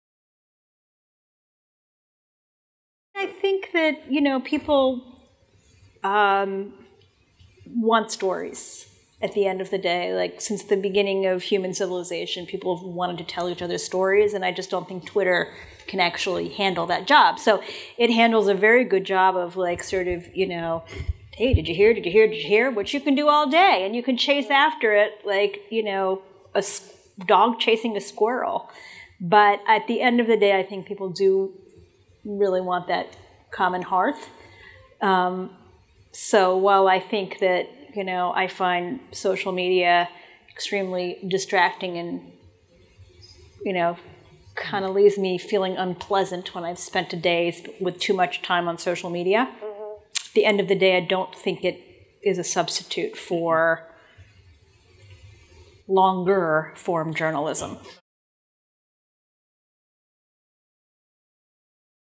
Twitter Won’t Suffice
Hate to break it to ya, but Twitter will never be a source for news. Sure, it helps you stay on top of headlines that are trending, but it does not provide content or any sort of long form journalism. People want to hear a story, it is impossible to write anything with context in 280 characters or less.